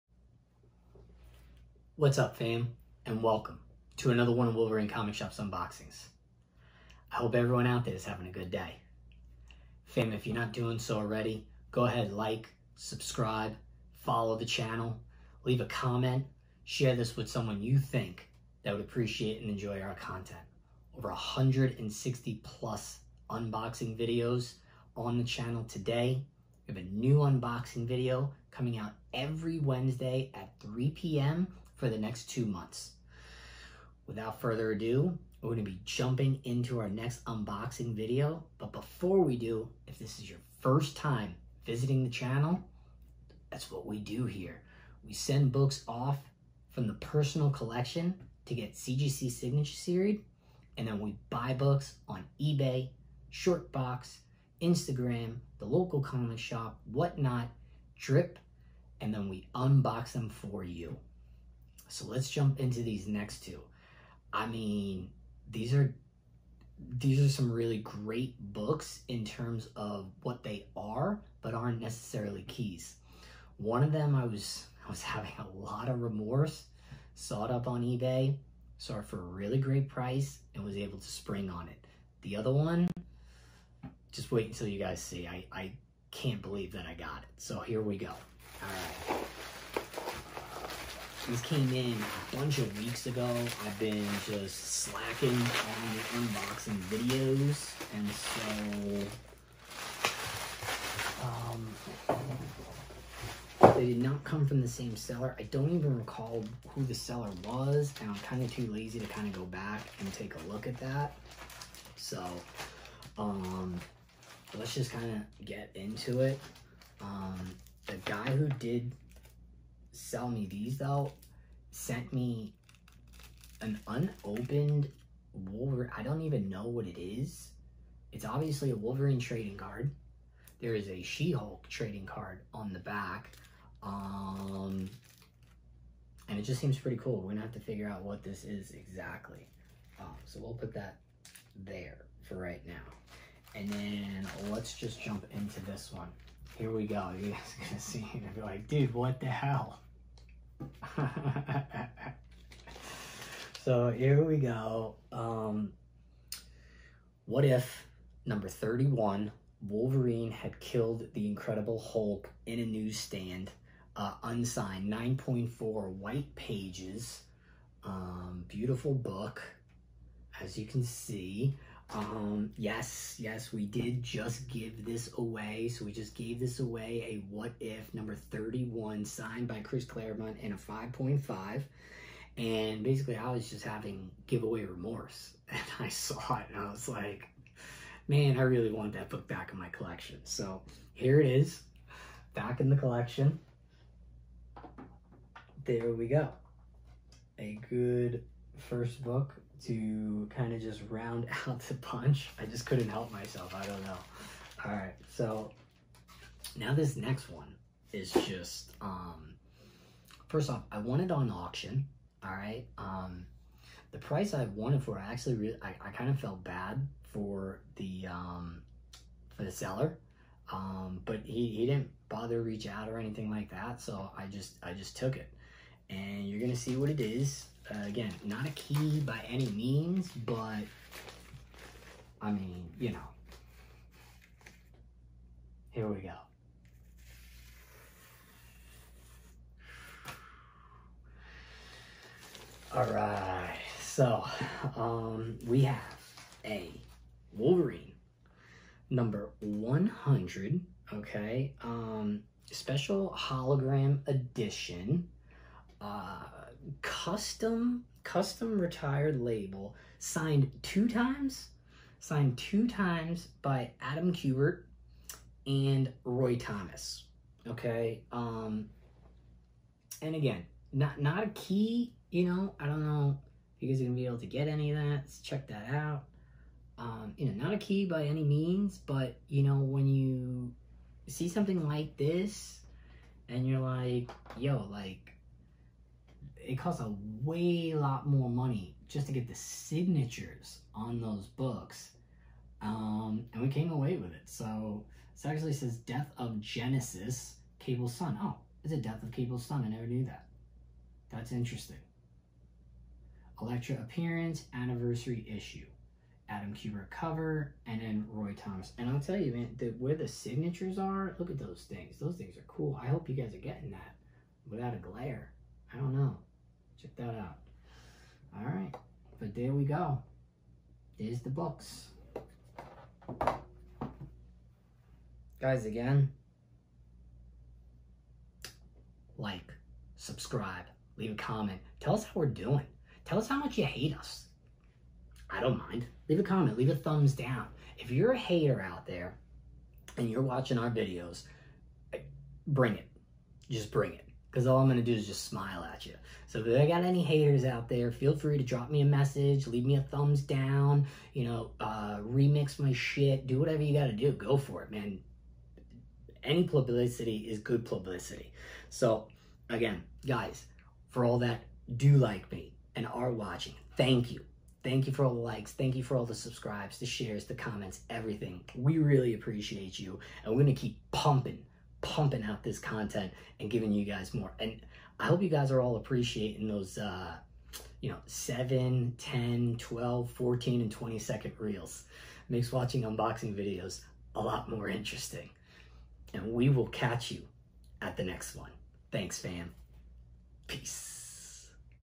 Wolverine CGC Signature Series Comic Book Unboxing
Join me for this Epic Wolverine CGC Signature Series Unboxing